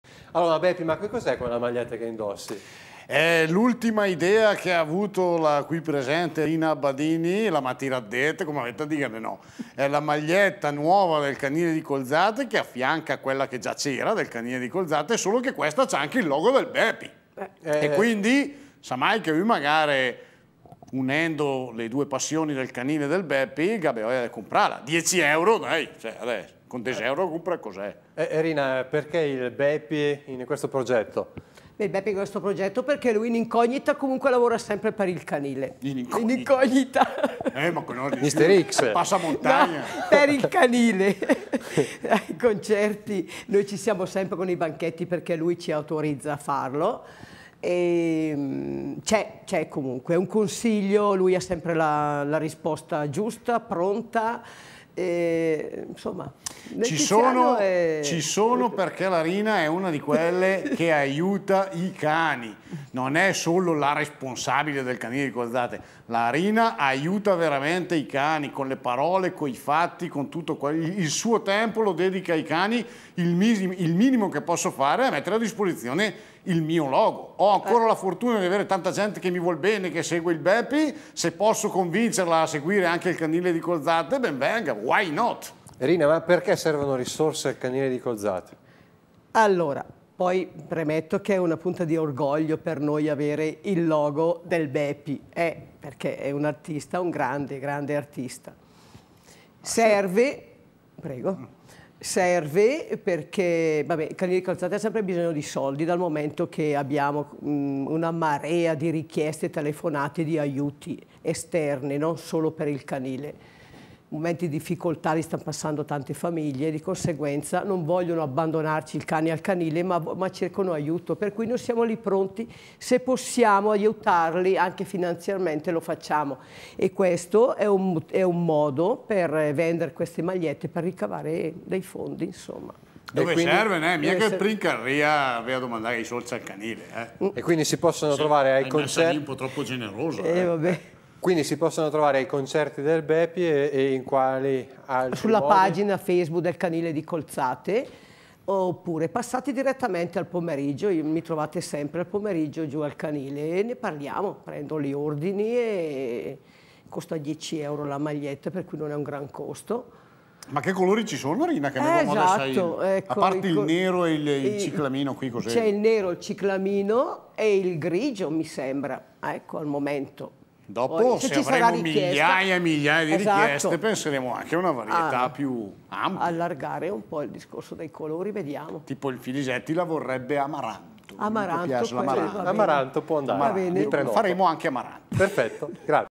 Colzate per il Canile una maglietta con il Bepi Antenna 2 TV 22052014
C'è anche il simbolo del Bepi sulla nuova t-shirt del Canile di Colzate. Il ricavato della vendita aiuterà a finanziare i progetti della struttura.